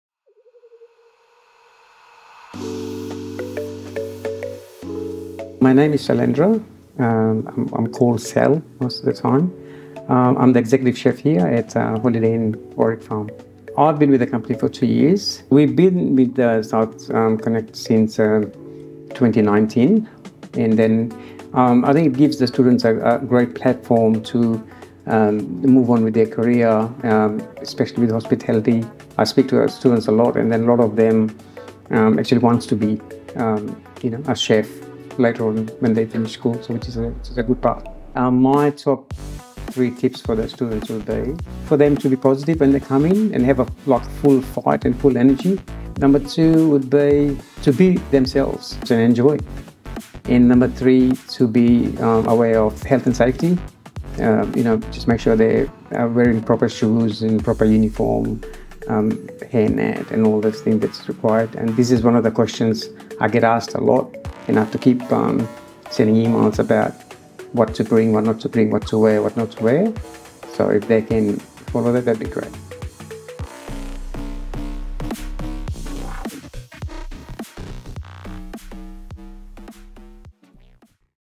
Host Employer Holiday Inn Warwick Farm Chef Salindra Cookery June 2024
Host Employer interview created by DigiVan (Skilled Creative) together with Entertainment students from Bossley Park High School.

To learn more about DigiVan and Skilled Creative, please